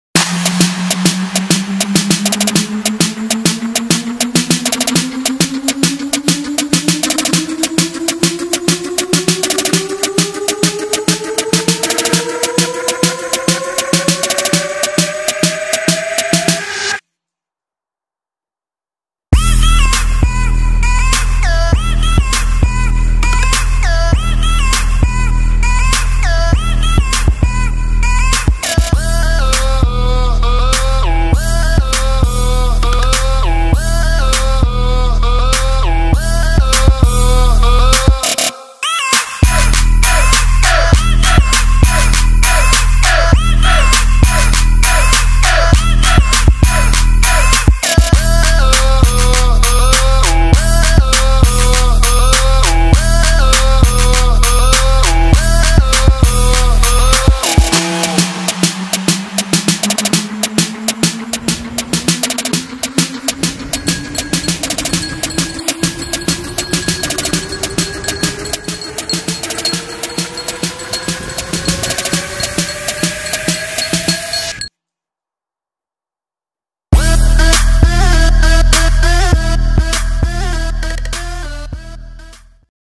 Turn down for a Merry Christmas!
A little sneak peak into what life is like for two of the hottest Bell Ringers to work the Underground Circuit. A lot of people say we should take our gift internationally, but we're just trying to take it one day at a time. Trying maintain humility while you're considered the greatest in the public eye is really tough, but we just take it one day at a time.